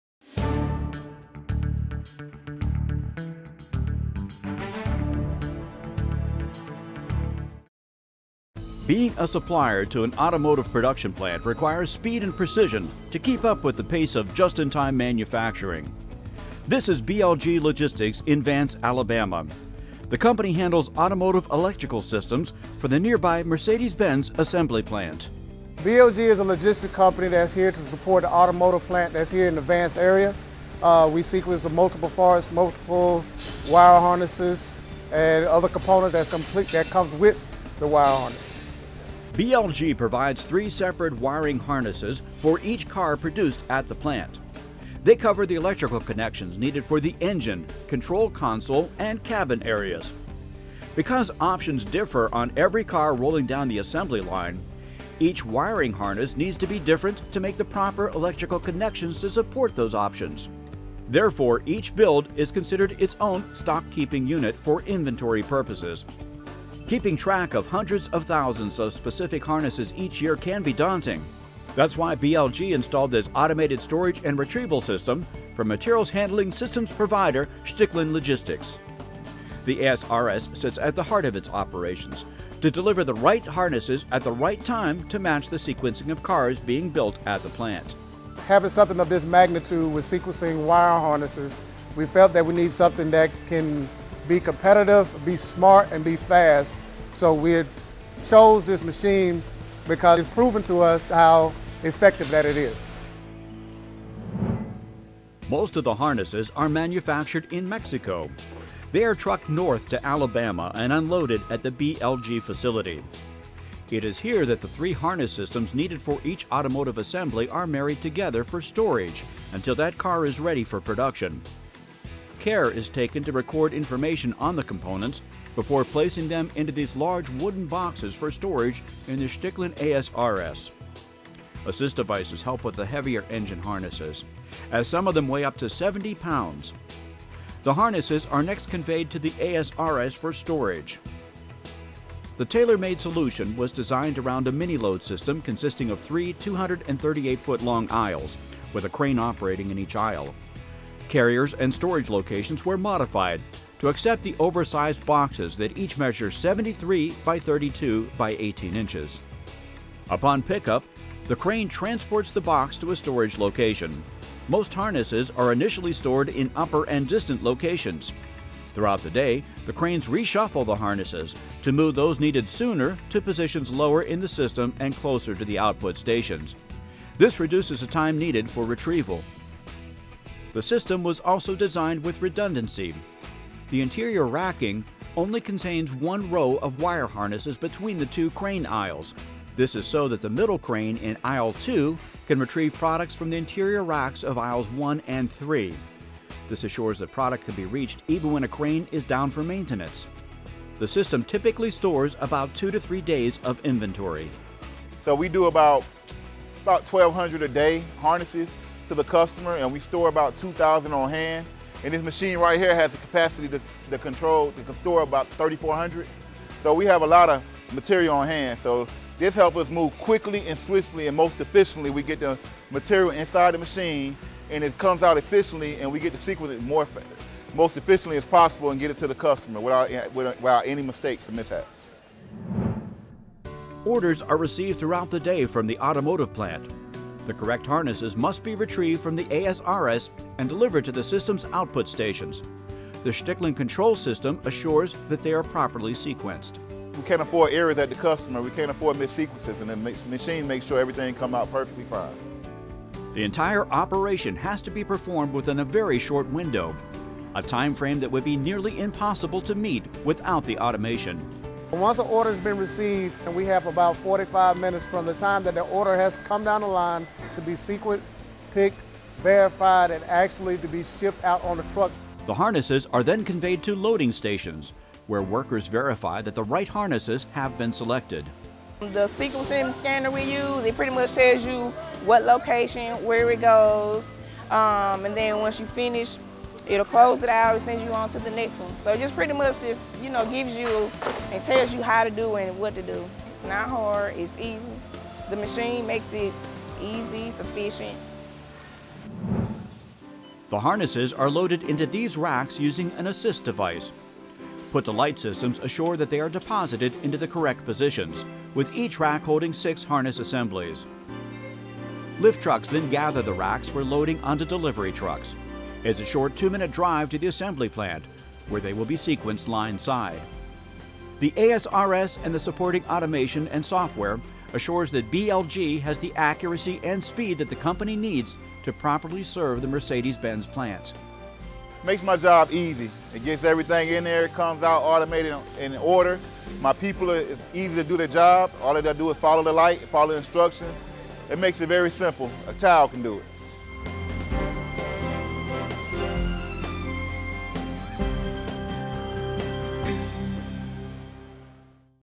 BLG Logistics: Harnessing automotive supply
Today's automotive assembly plants run on extremely tight production schedules. Materials and parts arrive at facilities just as they are needed on the vehicle assembly line. To add to this complexity, the many options available on today's automobiles mean that in many cases, the components are not the same for every car on the line. Suppliers must sequence the correct parts to match up with each individual vehicle being assembled.

At its 350,000-square-foot facility in Vance, Ala., BLG Logistics supplies wiring harnesses for the Mercedes- Benz assembly plant next door. The company receives the harnesses from manufacturers in Mexico, stores them for two or three days, and then sequences them to meet the exact specifications of cars on the line. It's a process that requires speed and precision—and one that must be performed within a very tight time frame.

"Once that order has been received, we have about 45 minutes to have the harnesses come down the line to be sequenced, picked, verified, and shipped to the plant," explains Jamaal Roberts, manager of operations at BLG.

Work at this facility would be nearly impossible without automation and software systems to drive accurate processing. At the heart of its operations, BLG relies on an automated storage and retrieval system (AS/RS) supplied by Stoecklin Logistics. The threeaisle system is based on a mini-load system that was modified to accommodate the large wooden boxes that hold the harnesses. The boxes measure 73 by 32 by18 inches, requiring larger carriages and right-sized racking locations to hold them.

To assure uptime, the interior racking within the AS/RS only contains one wiring harness between the two crane aisles. This is so that the middle crane in Aisle 2 can retrieve products from the interior racks of Aisles 1 and 3. The system has capacity for 3,400 harness sets in a footprint of only 50,000 square feet. At any given time, there are between 2,000 and 3,000 sets in storage. The additional capacity allows for growth and a continuous reshuffling of inventory, so that harnesses needed soon are re-deposited closer to output stations to reduce retrieval time.

SMART & FAST
Any delays in processing at BLG could stop the line at Mercedes- Benz. That's why it's critical that accuracy and speed are assured. Each car rolling down the line at Mercedes-Benz actually requires three different harness assemblies to meet the electrical connections in the engine, control console, and cabin interior. BLG uses the storage boxes to combine the harnesses needed for each car model based on the build plan. The harnesses then remain together in storage until needed for assembly next door. At that time, information is sent to the warehouse management system, which then coordinates with the Stoecklin warehouse control system to direct the cranes to retrieve the needed harnesses. The system handles 70 boxes in and 70 boxes out per hour.

The cranes deliver boxes of harnesses onto conveyors for transport to processing stations. Workers use scanners to verify that the correct harnesses have been delivered. They then use a put-to-light system to load the harness sets into metal racks for transport to the automotive plant, where they will arrive just before they're needed for assembly. The facility processes about 1,100 harness sets each day, depending on the plant's requirements, and 1,400 sets at peak production. The automation assures that BLG can meet the fast-paced demands of the plant continuously.

"The Stoecklin AS/RS is very dependable. We can run all day," says Roberts. "We chose this machine because it allows us to be competitive, smart, and fast."

For more information on Stoecklin Logistics, go to stoecklin.us.